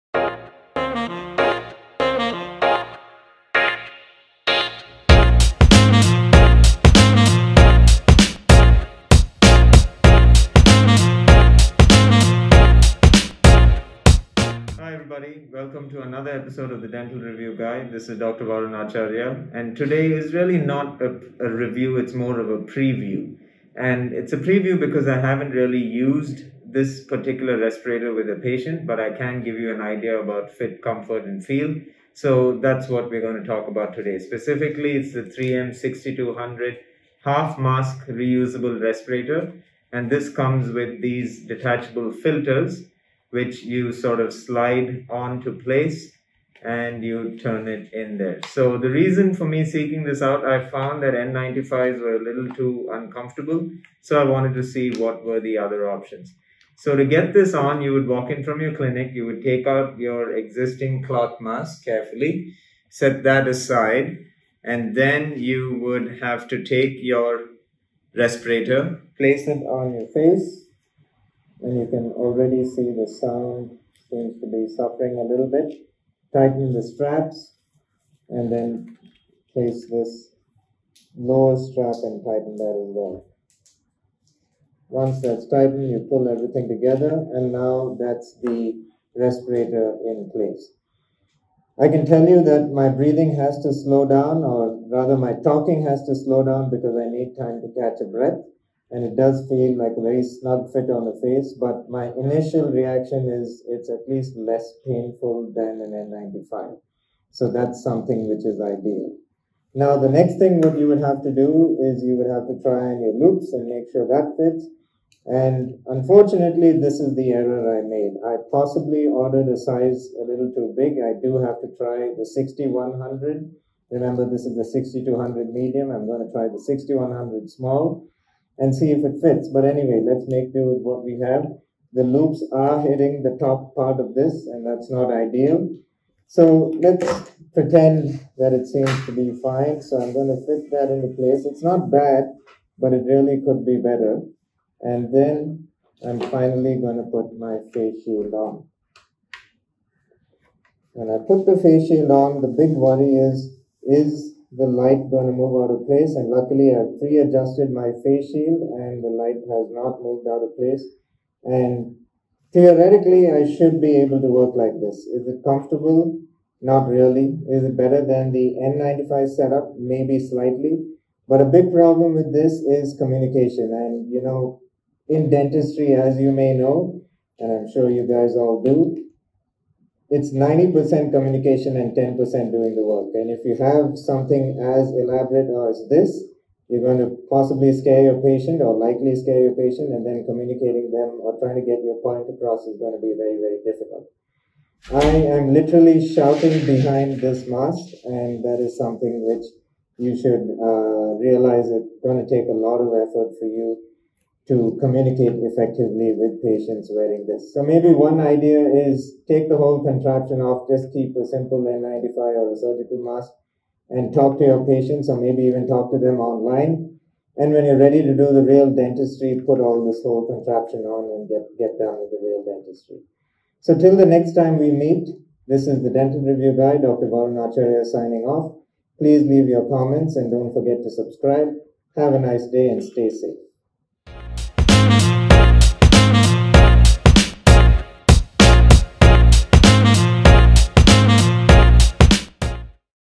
Reusable Respirators - Initial impression
While we wait for official evidence-based information on what works best for protecting ourselves and our staff from dental aerosols, The Dental Review Guy previews the half facepiece reusable respirator from 3M. Check it out if you're considering this option for yourself or your team.

One of the concerns is this type of respirator does NOT filter exhaled air. Please see 3M's latest advisory on their respirators

Facebook/YouTube/Twitter: The Dental Review Guy